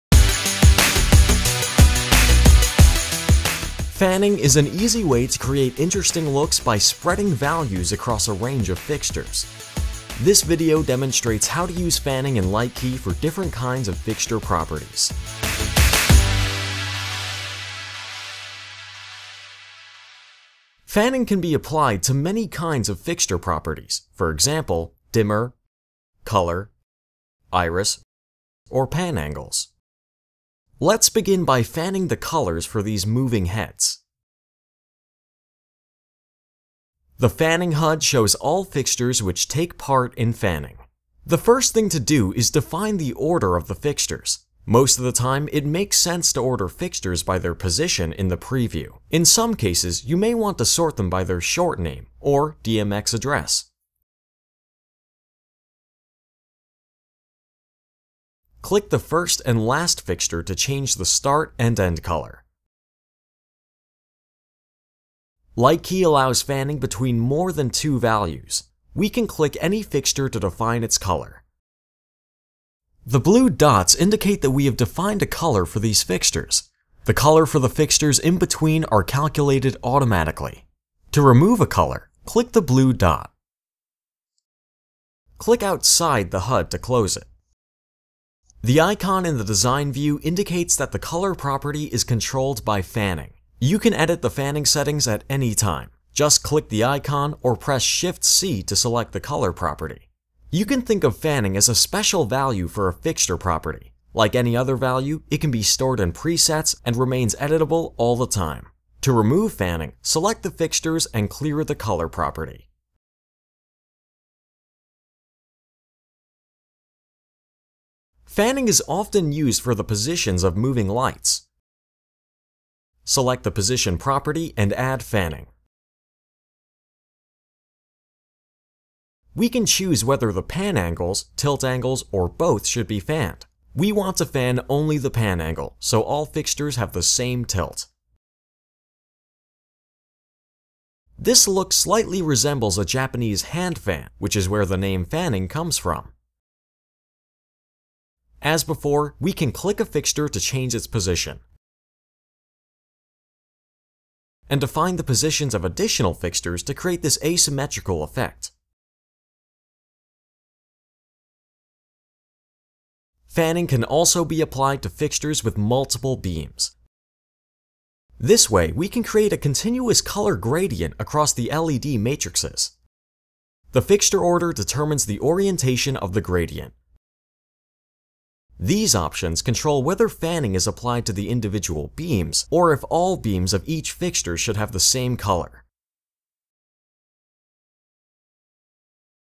How to Use Fanning
Fanning is an easy way to create interesting looks by spreading values across a range of fixtures. This video demonstrates how to use fanning in Lightkey for different kinds of fixture properties.